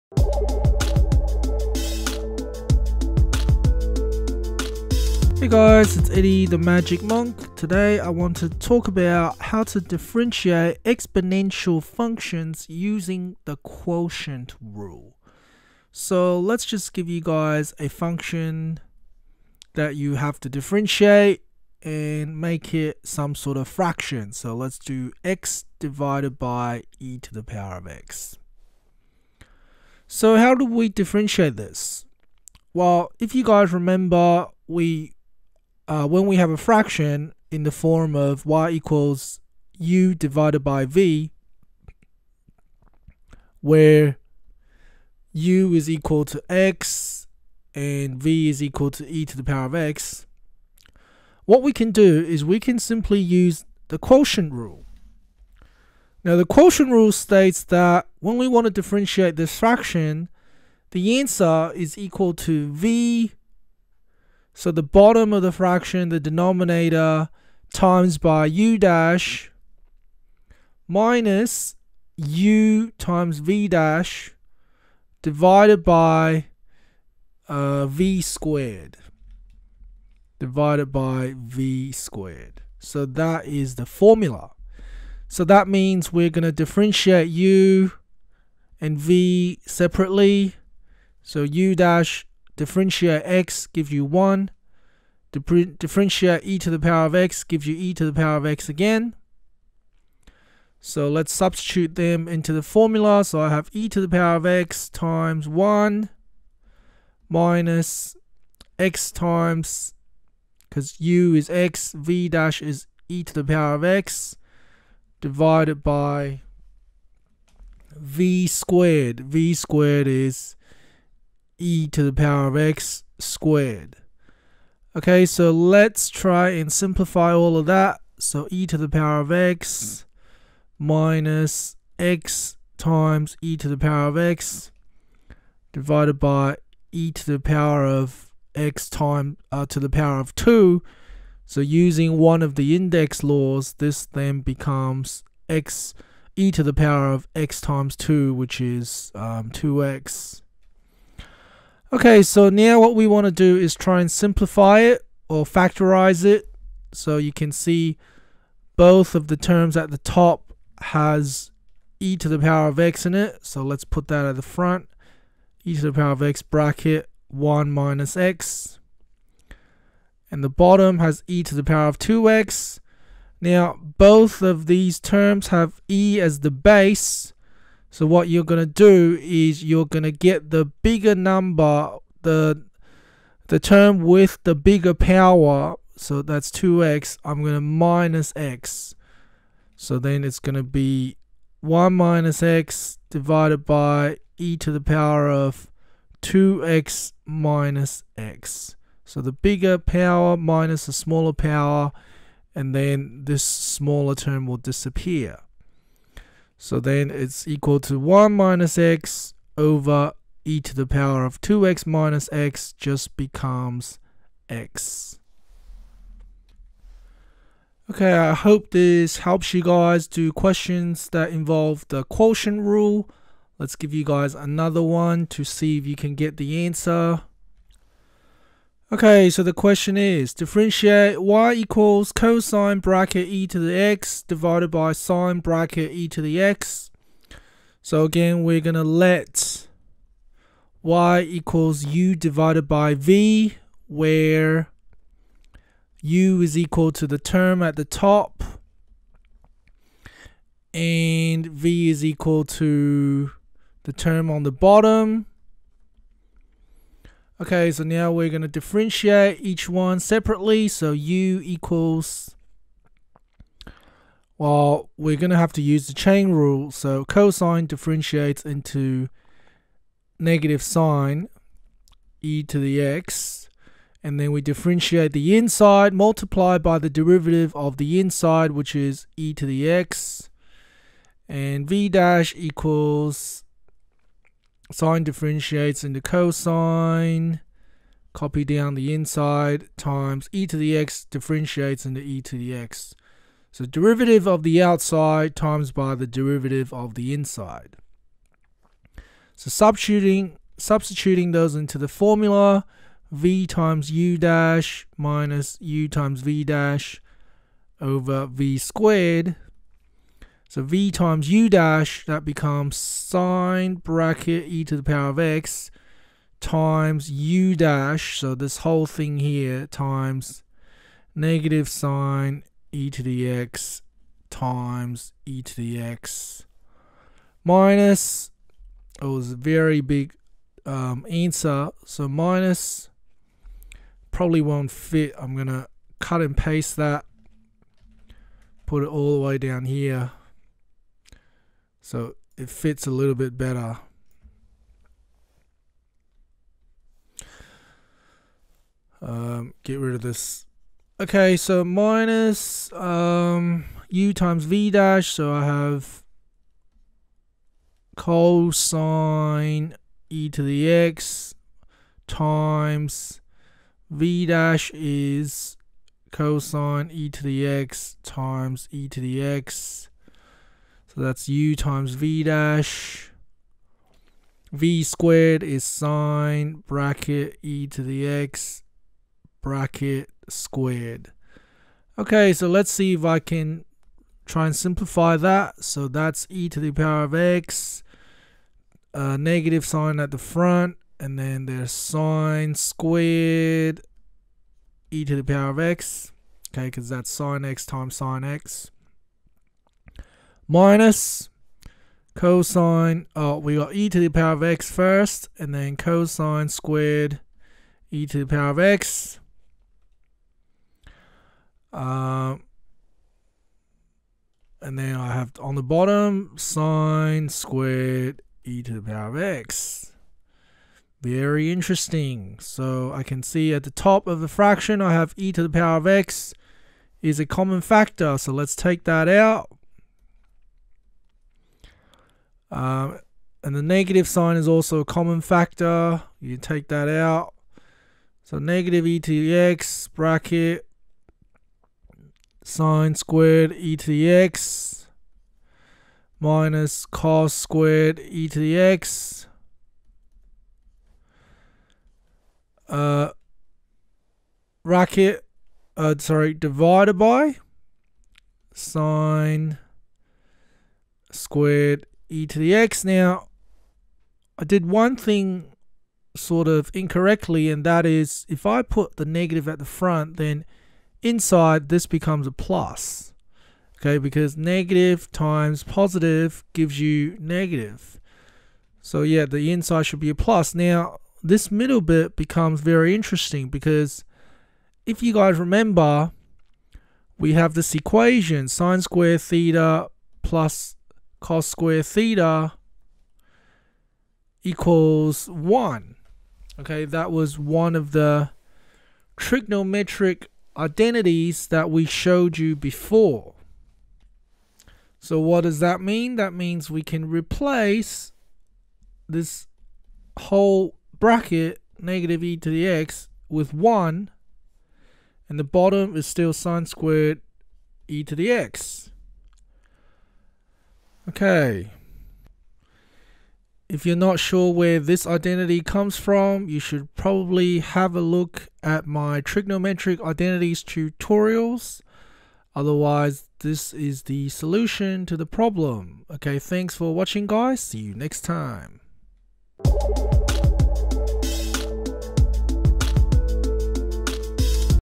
Math B - Differentiate Exponential Functions using Quotient rule
In this video we talk about how to differentiate exponential functions that require the quotient rule.